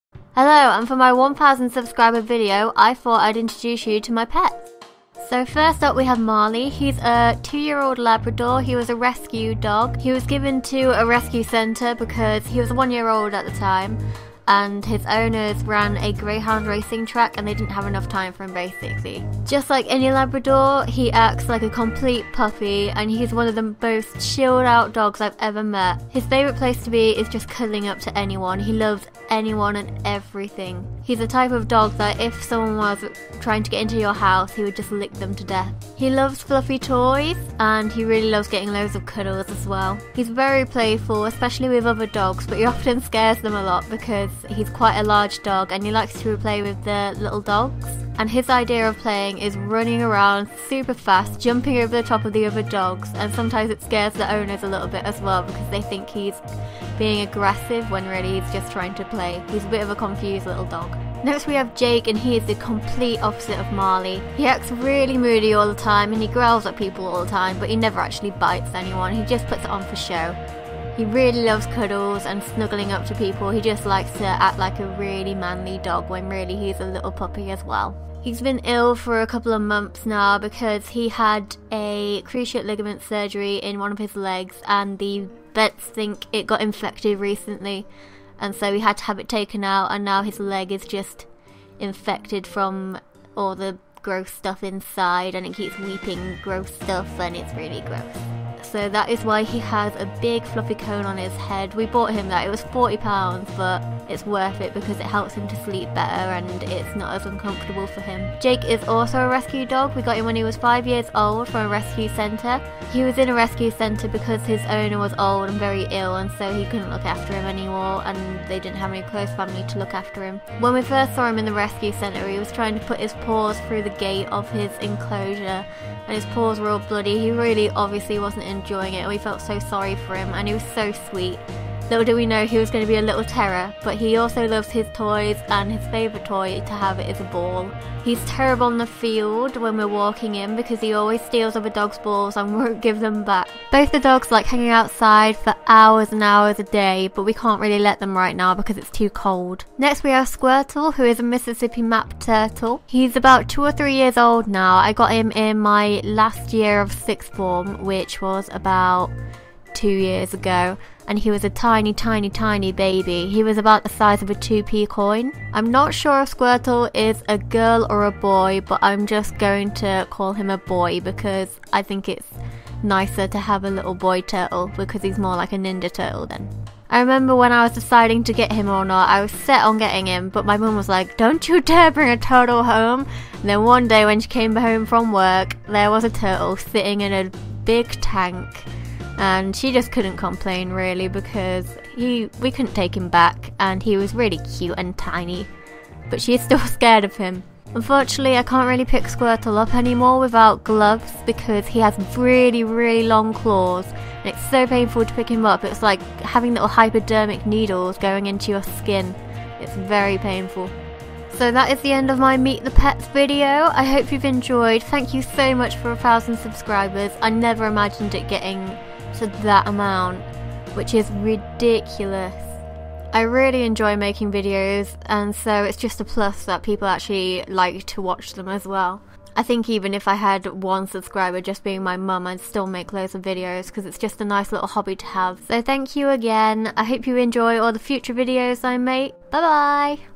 Meet My Pets! (1000 Subs)
Since quite a lot of people comment on my dogs, I thought I'd tell you a little about them and my other pet :)